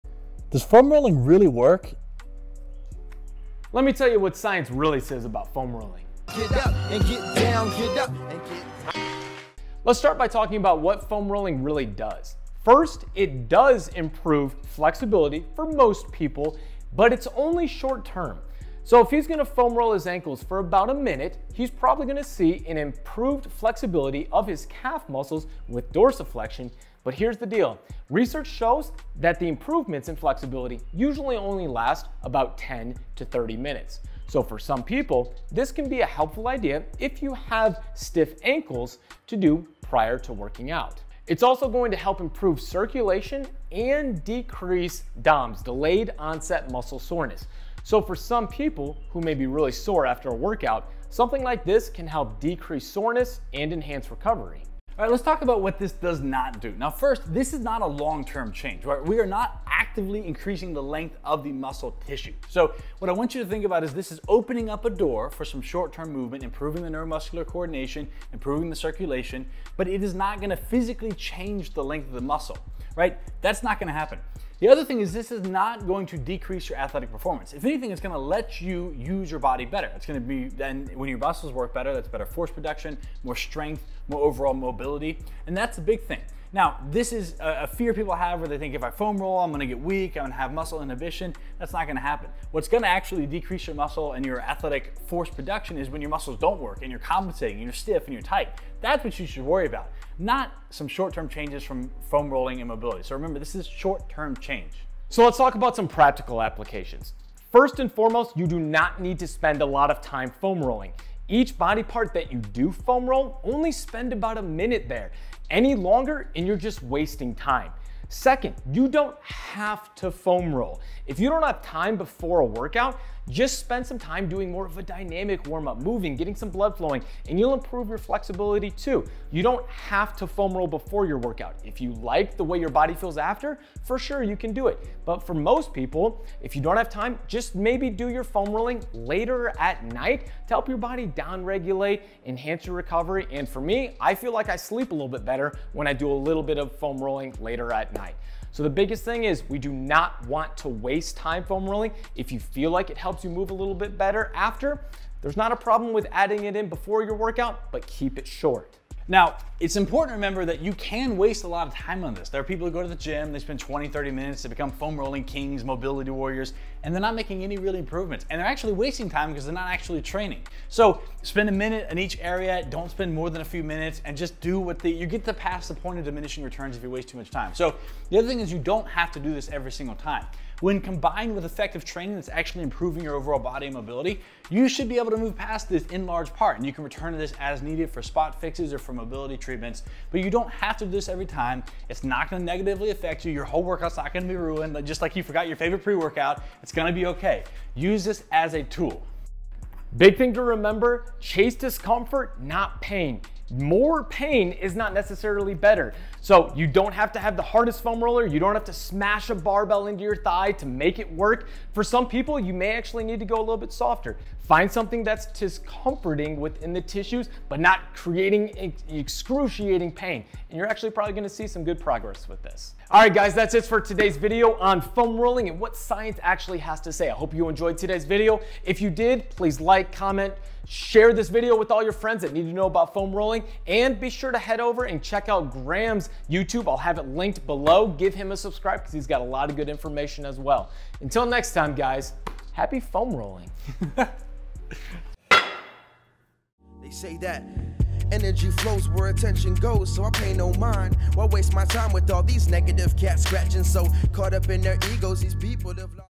Does FOAM ROLLING Work?
Today you'll learn the science behind foam rolling, collaboration with @thebarefootsprinter.